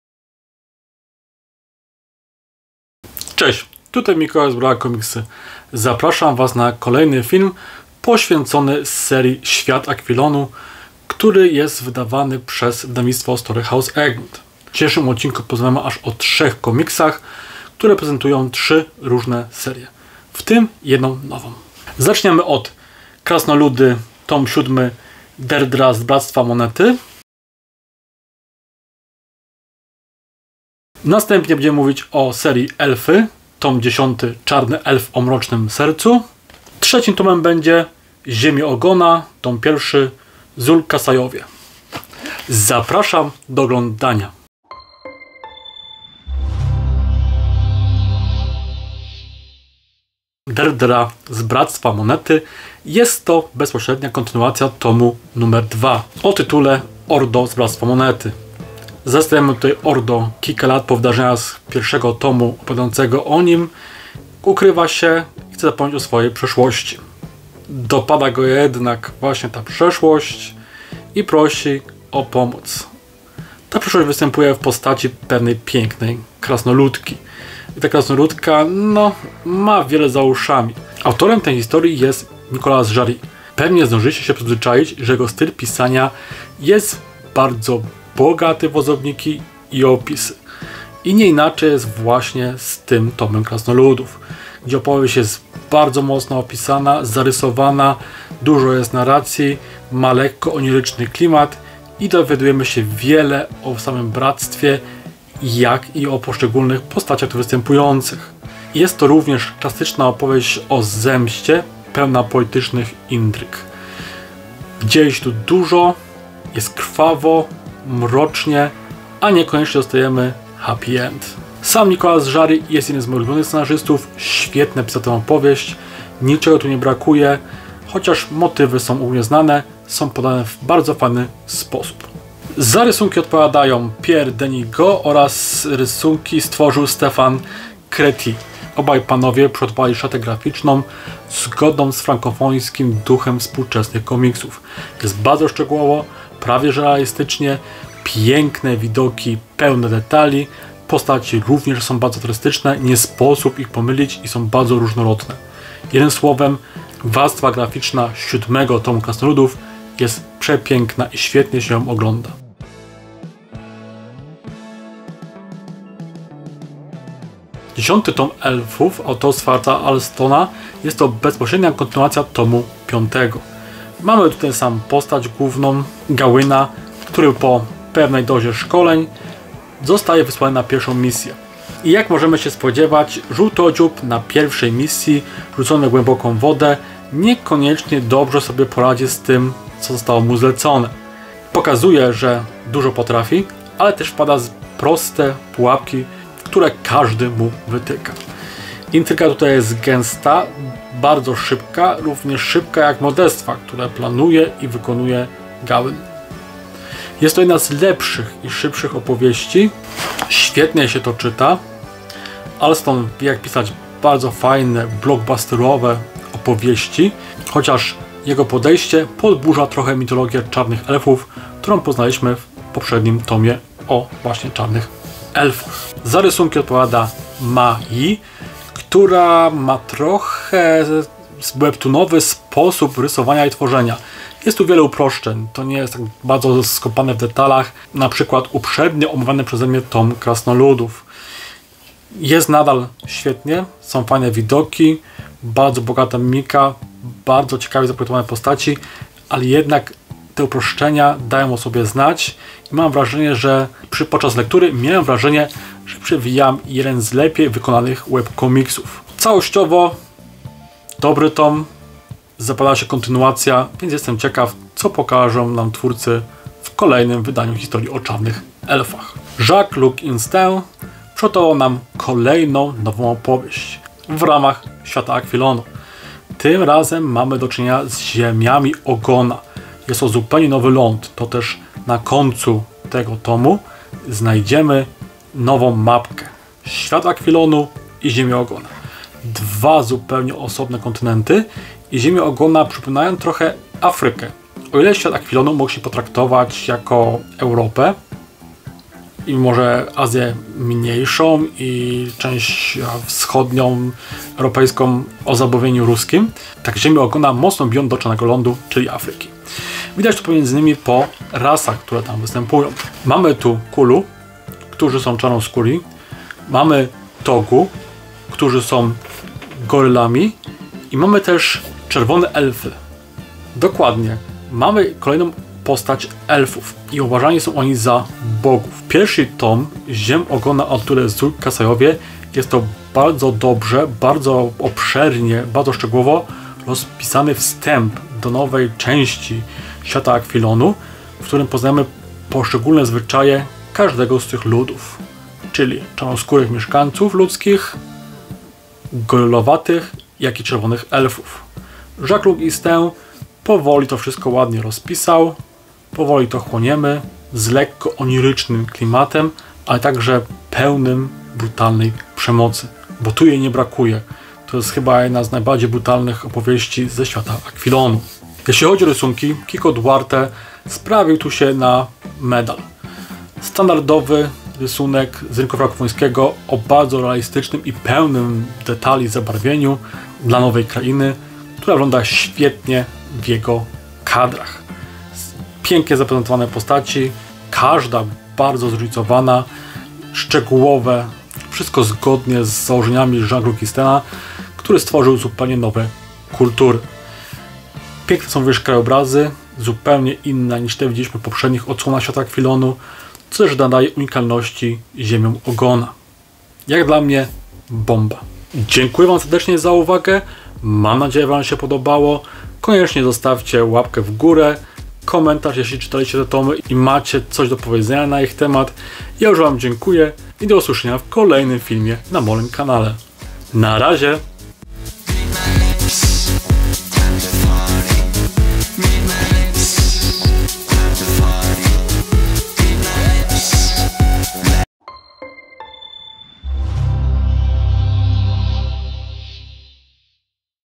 Świat Akwilonu 10: Krasnoludy, Elfy, Ziemie Ogona
Komiksy ze Świata Akwilonu na dobre zadomowiły się na polskim rynku komiksowym. Kolejne części cyklu ukazujące skrawki ogromnego, fantastycznego świata rozpalają wyobraźnię wielu czytelników, którzy otrzymują intrygujące pomysły.

W niniejszym filmie omawiam kolejne tomy:

00:00 Wstęp
00:50 Krasnoludy. Tom 7. Derdhra z Bractwa Monety
02:47 Elfy. Tom 10. Czarny Elf o mrocznym sercu
04:29 Ziemie Ogona. Tom 1. Zul Kassajowie